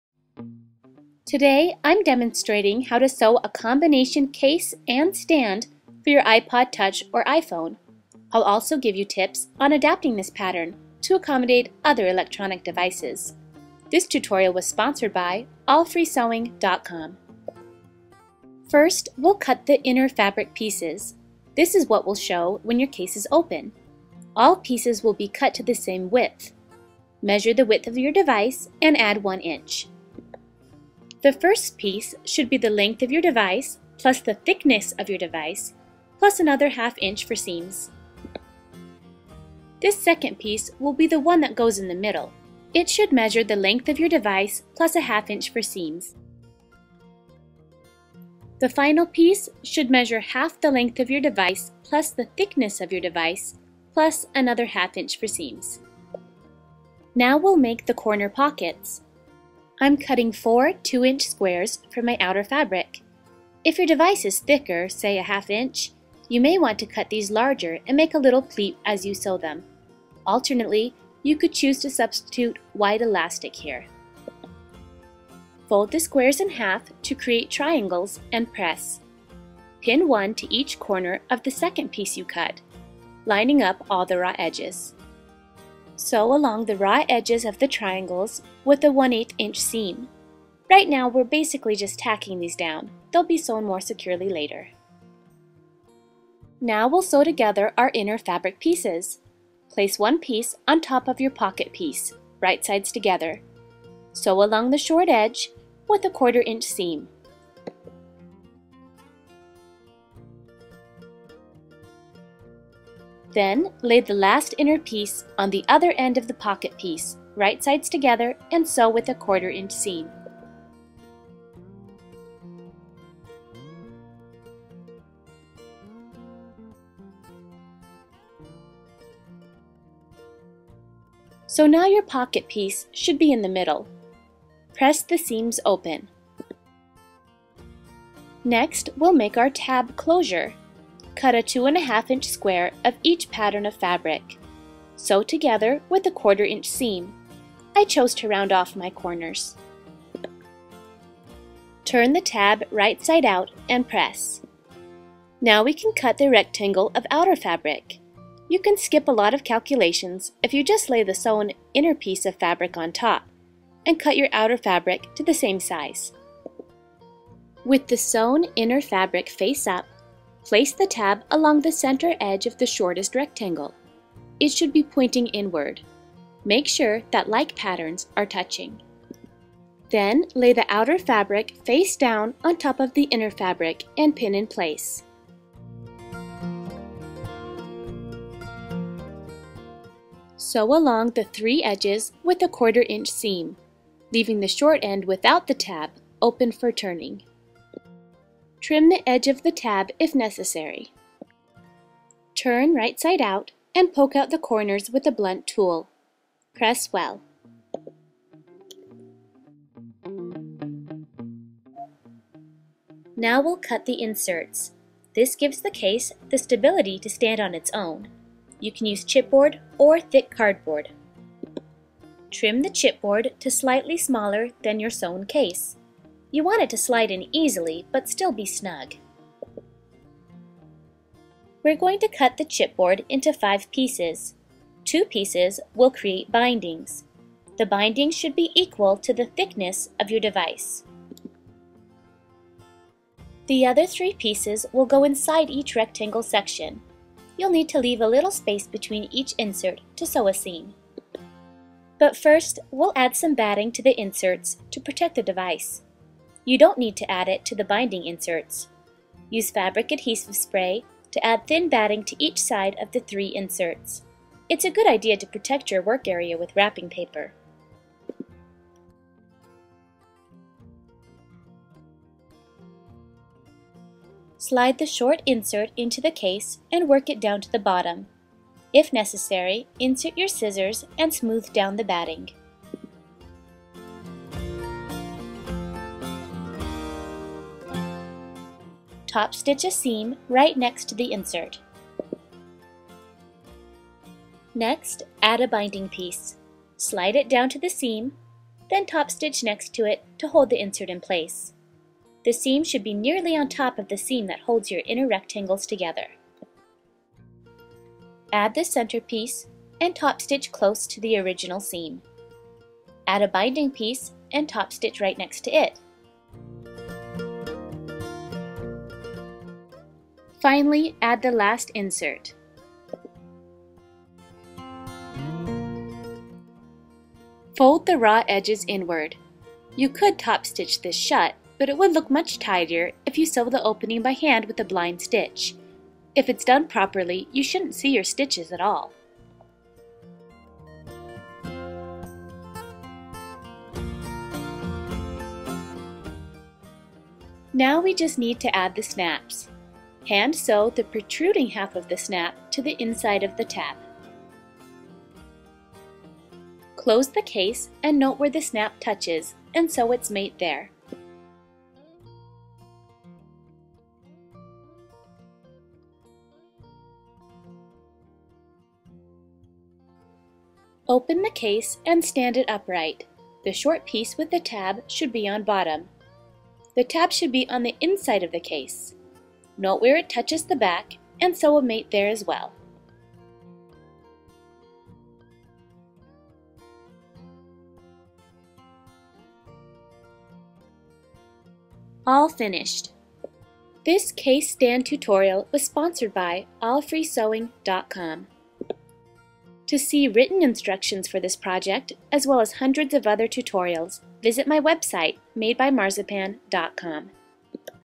Sew an iPod Touch / iPhone Case Stand
This combo Case / Stand is lightly padded to protect your device, snaps shut like a wallet, and stands up on its own. Measurements included for iPod Touch, with formulas for adapting to your e-reader or other device. Try making one for your Kindle or iPad.

Pattern for personal use only. If you're interested in making these for retail sale, please see my website for info on becoming an Authorized Seller.